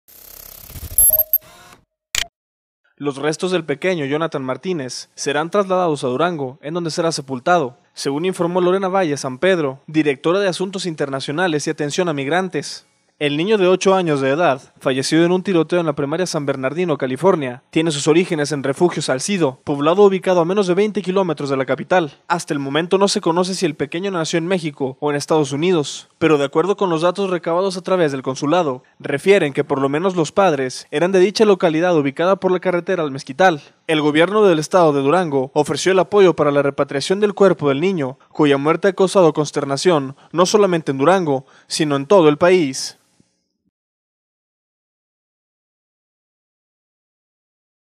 Niño víctima de tiroteo en EU será sepultado en Dgo
Los restos del pequeño Jonathan Martínez, serán trasladados a Durango, en donde será sepultado según informó Lorena Valles, directora de Asuntos Internacionales y Atención a Migrantes.